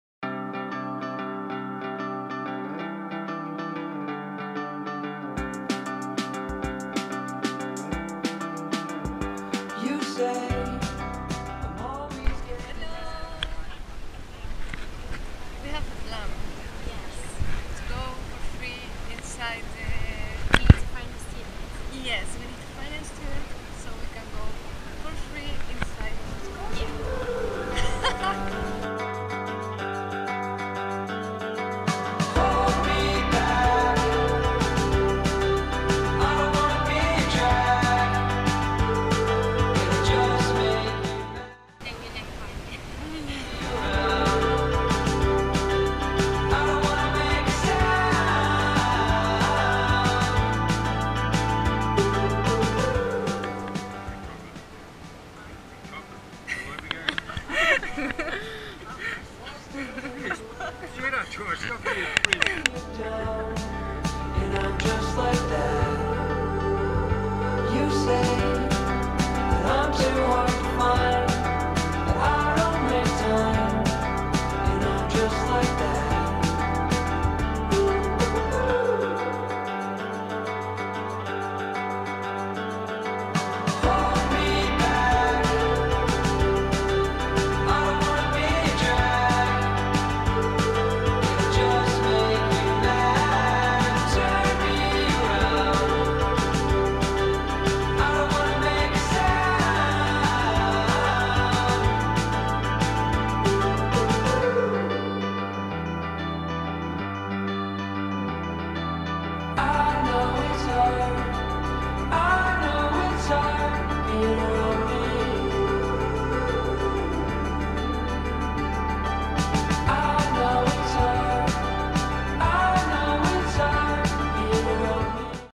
Challenge 1. Trinity College (Dublin)
What about going inside the Trinity College library for free? Ana and Elisa try to accomplish this mission during their first day in Dublin. Have they made it? Check out the video and discover it ;)